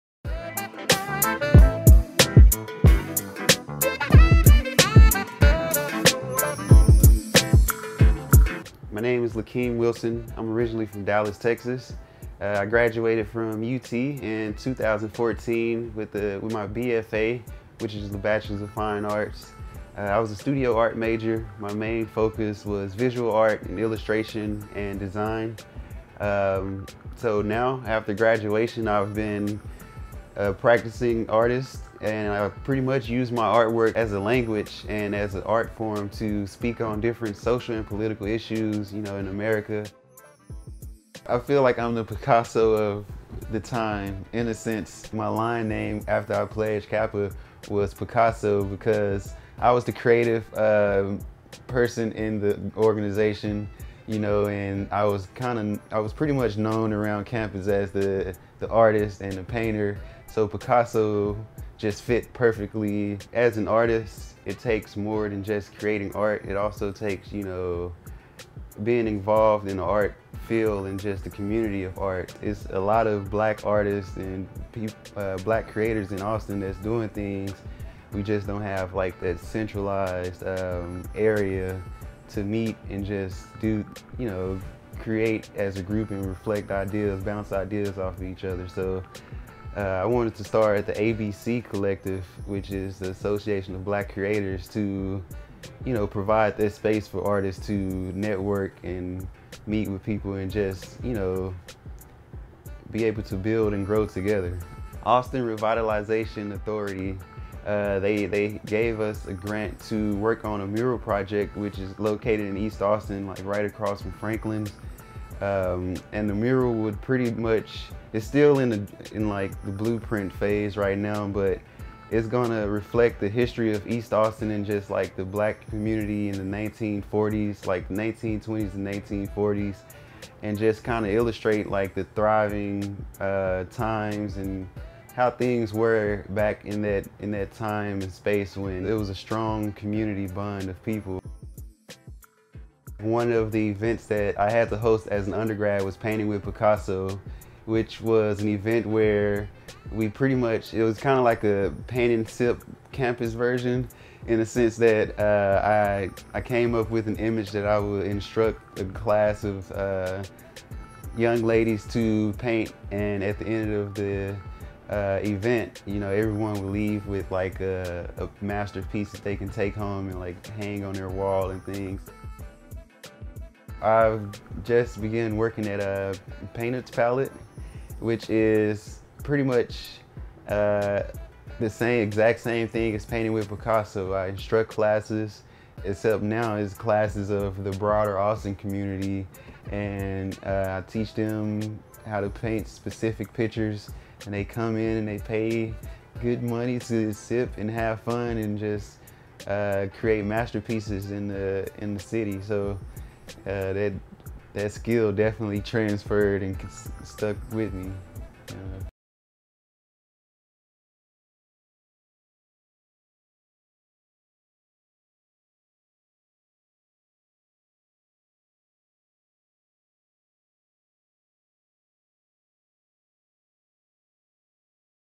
Suite Life Selection Spotlight: Lakeem Wilson
In this interview, Lakeem talks about how his artwork addresses societal issues and how this has shaped his perception and outlook in today’s society.